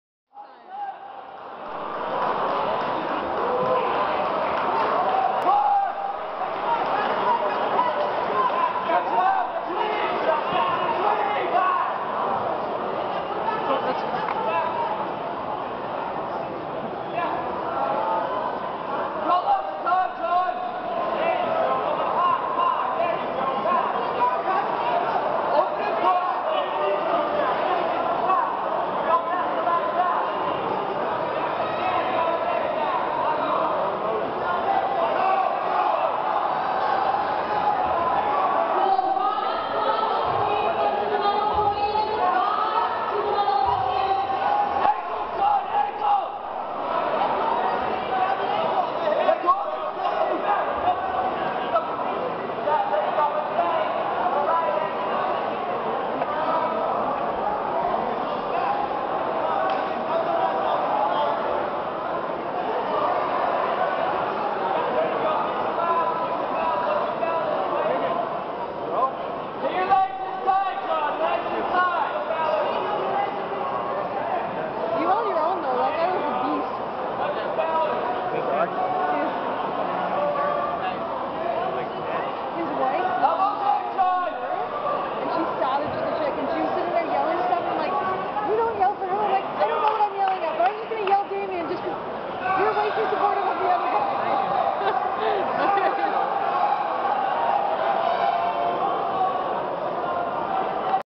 Jon May 5
Jon's Match at the BC Provincials Tournament May 5th 2012